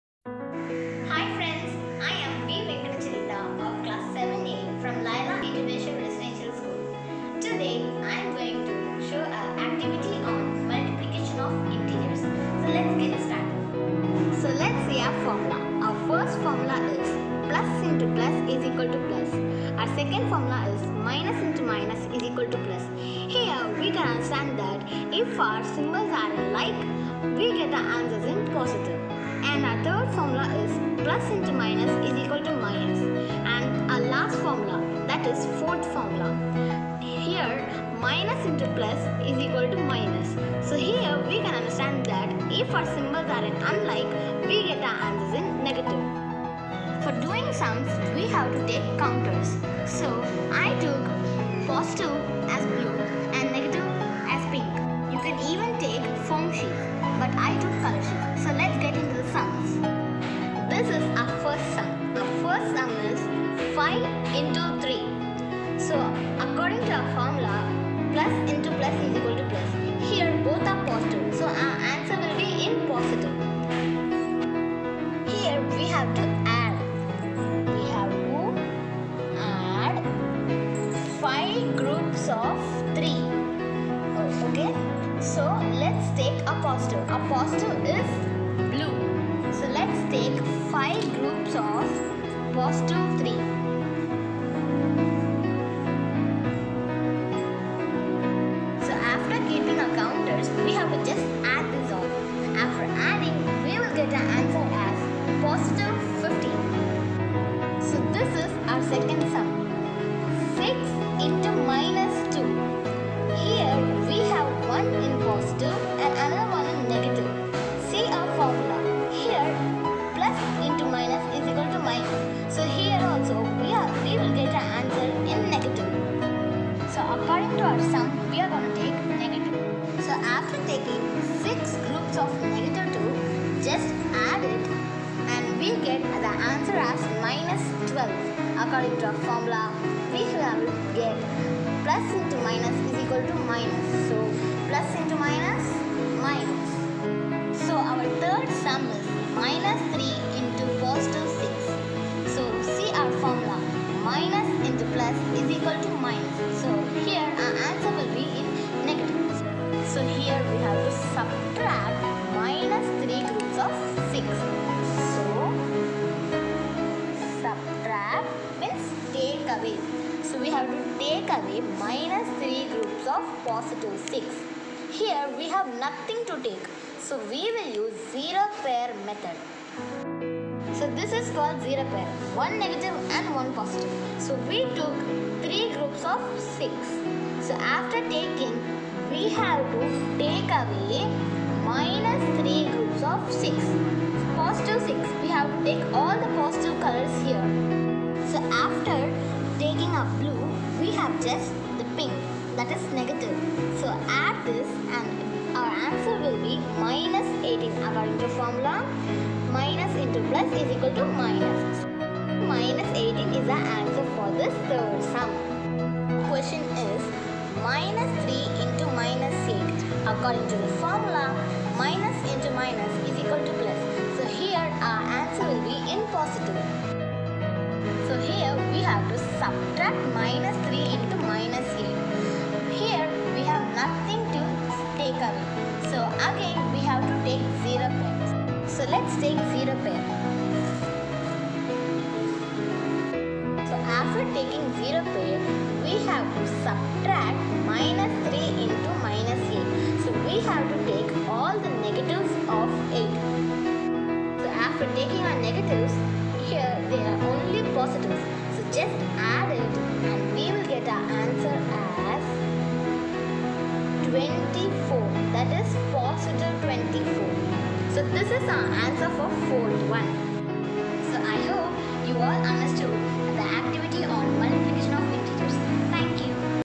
Activity on Multiplication of Integers by a student of LIRS,Palanchur
Colorful Presentation by Miss.Venkata Charitha of Grade 7